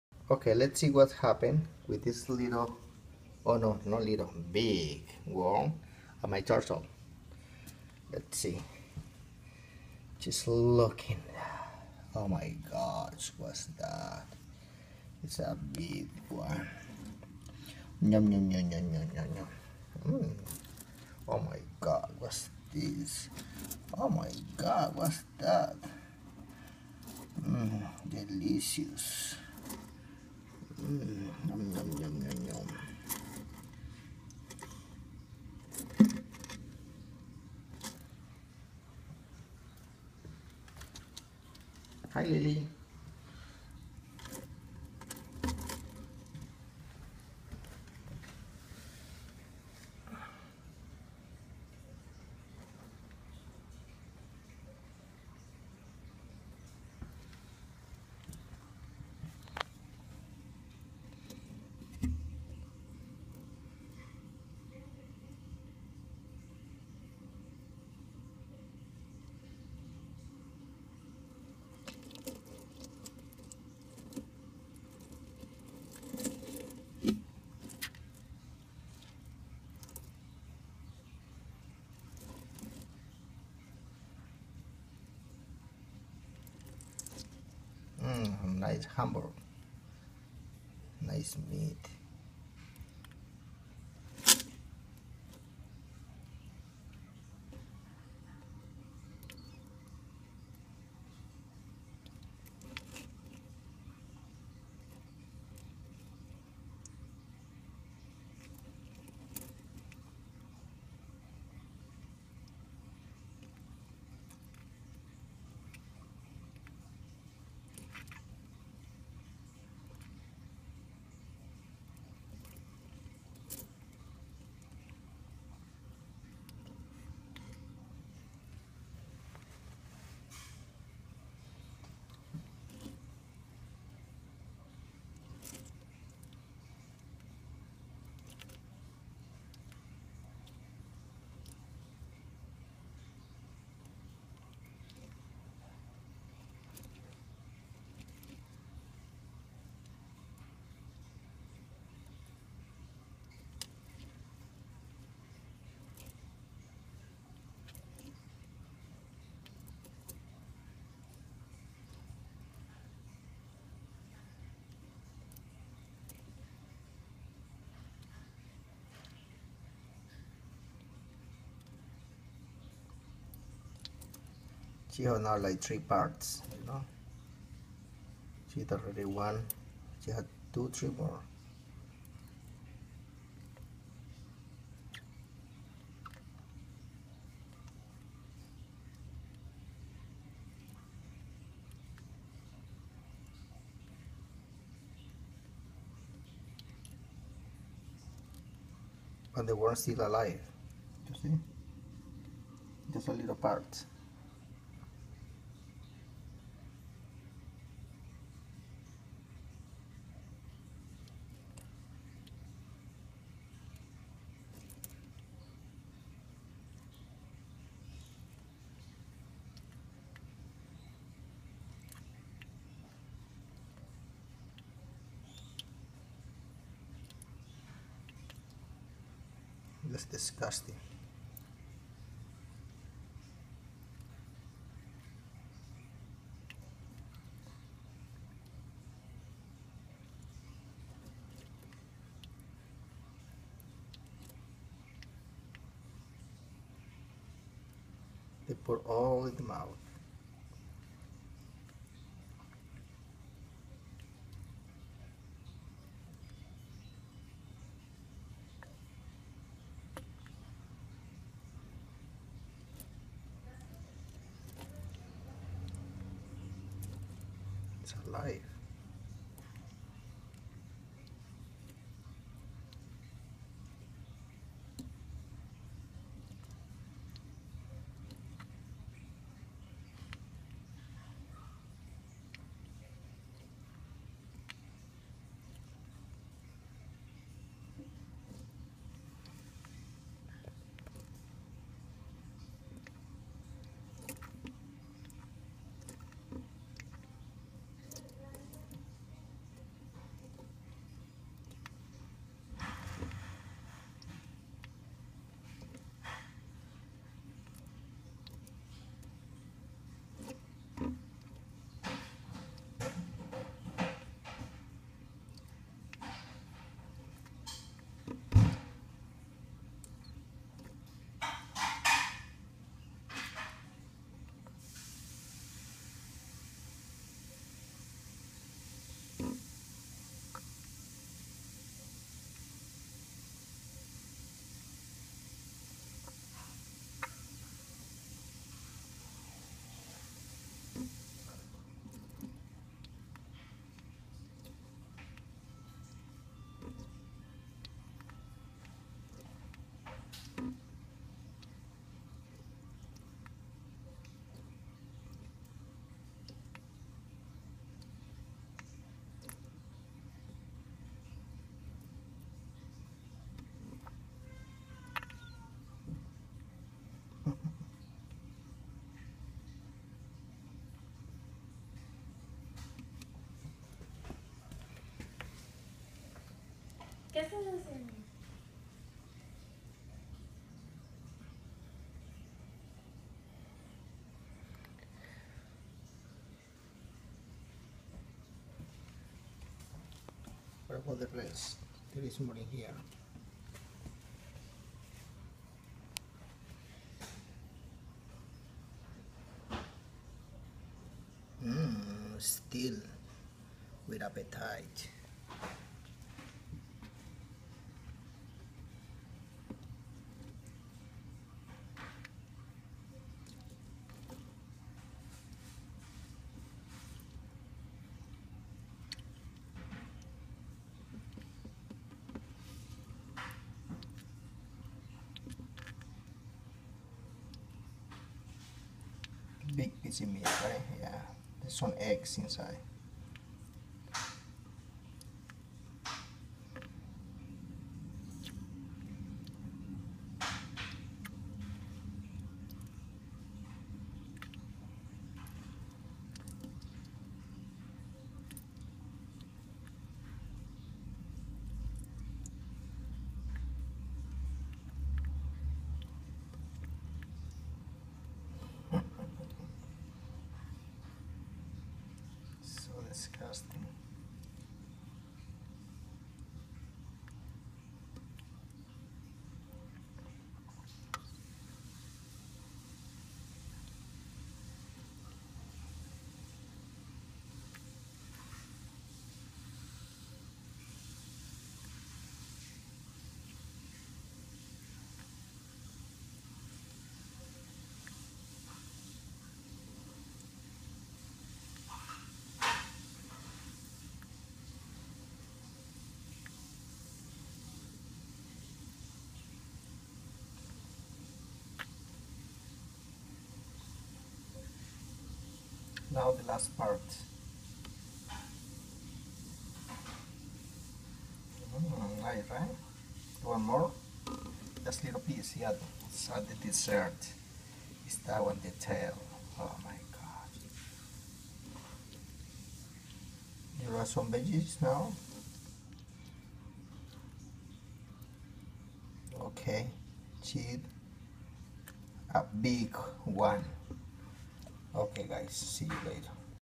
turtle eating red worm
In box turtle eating a big red juicy
worm